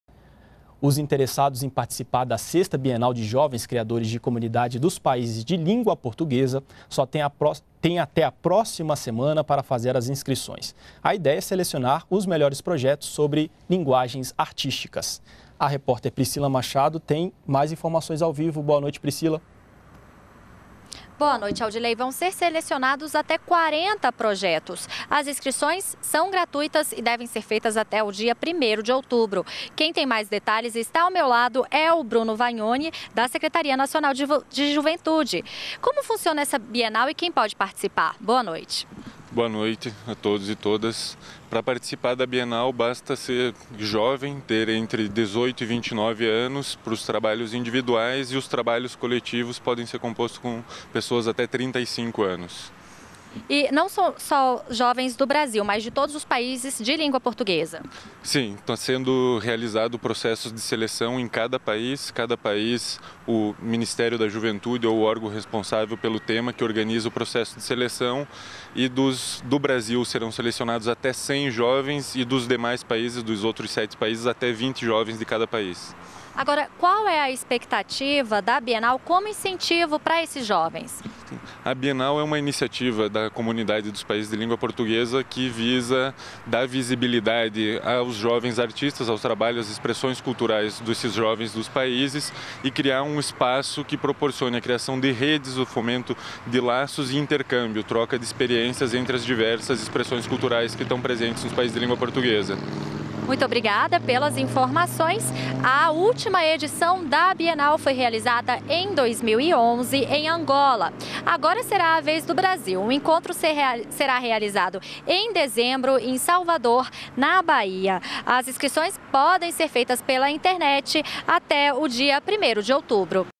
Bienal seleciona projetos de jovens sobre linguagens artísticas
NBR NOTÍCIAS - 26.09.13: Estão abertas as inscrições para a 6ª Bienal de Jovens Criadores da Comunidade dos Países de Língua Portuguesa. Serão selecionados até 40 projetos individuais e coletivos sobre linguagens artísticas de jovens de todos os países que falam a língua portuguesa. As inscrições podem ser feitas até o dia 1º de outubro. O assistente de relações internacionais da Secretaria Nacional de Juventude, Bruno Vanhoni, explica os detalhes da seleção. O evento será sediado pela primeira vez no Brasil, entre 3 e 7 de dezembro, em Salvador (BA). As inscrições são gratuitas e devem ser feitas pelo site do evento